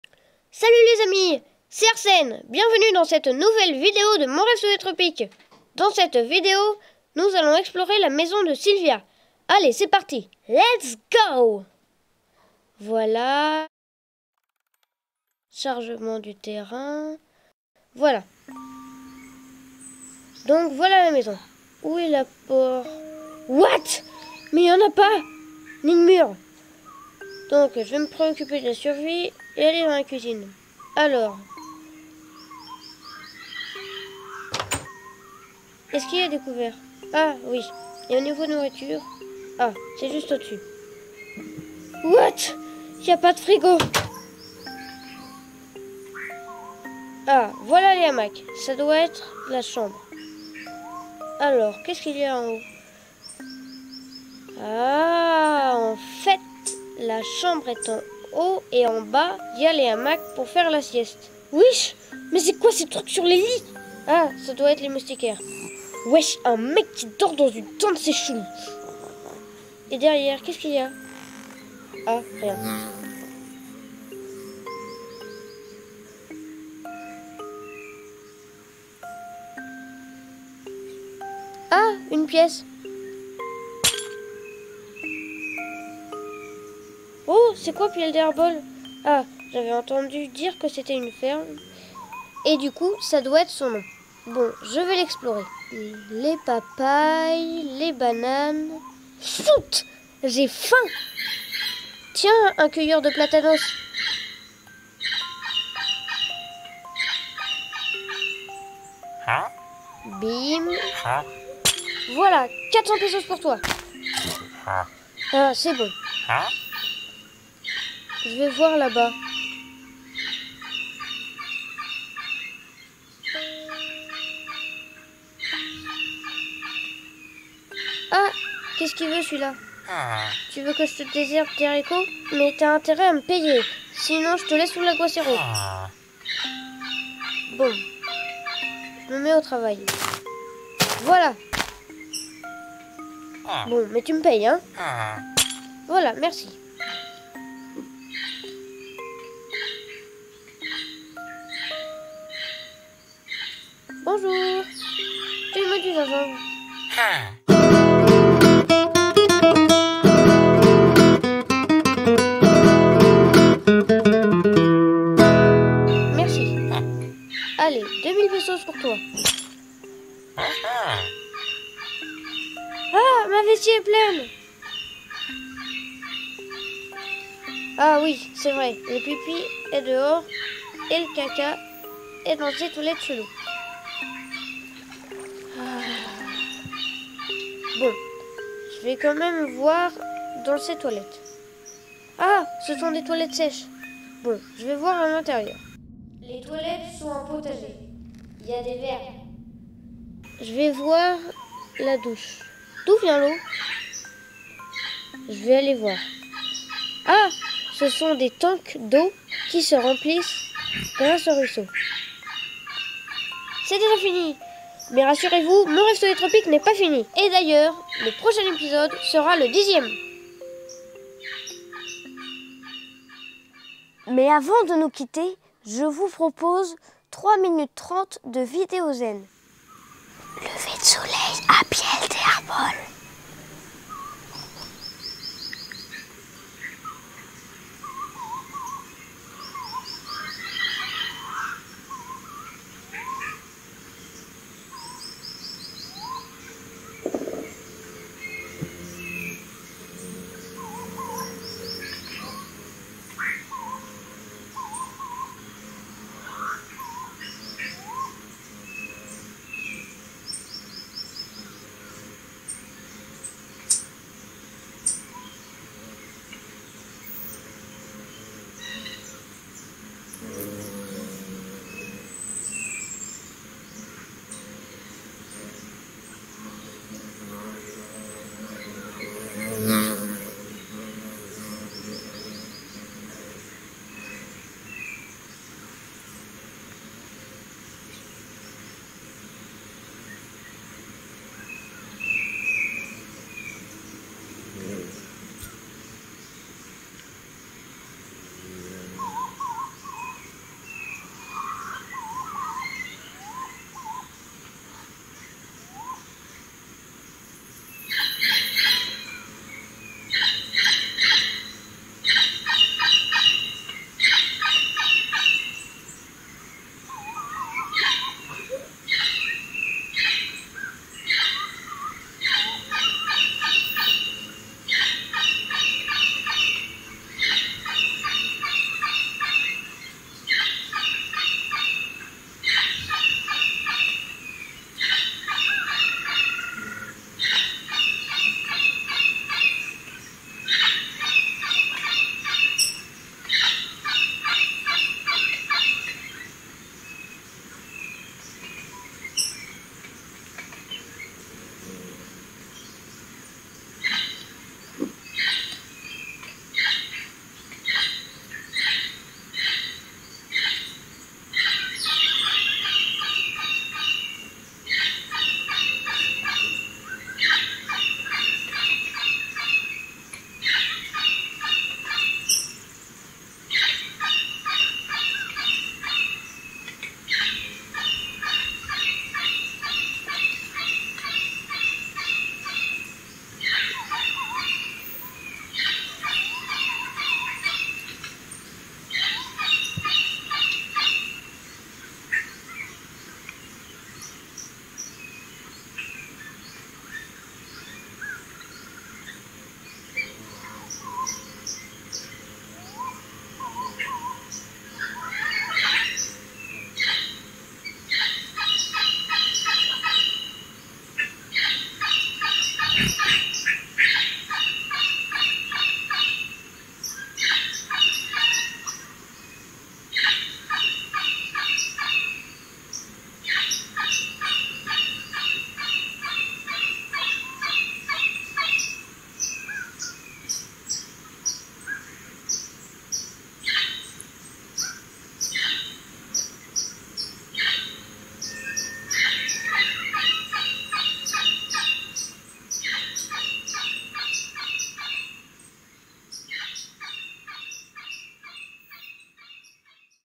Mon-Rêve sous les Tropiques #9
Le tour d'Arsène est revenu, et il a choisi de vous raconter l'endroit où nous vivons à la manière d'une "vidéo Minecraft". Vous ne connaissez pas ce jeux, ni les multitudes de youtuber qui produisent ce genre de vidéos ? Voici une bonne manière d'en avoir un aperçu, somme-toute assez fidèle. Et si vous trouvez ça trop rapide, nous vous offrons à la fin un petit surplus détente...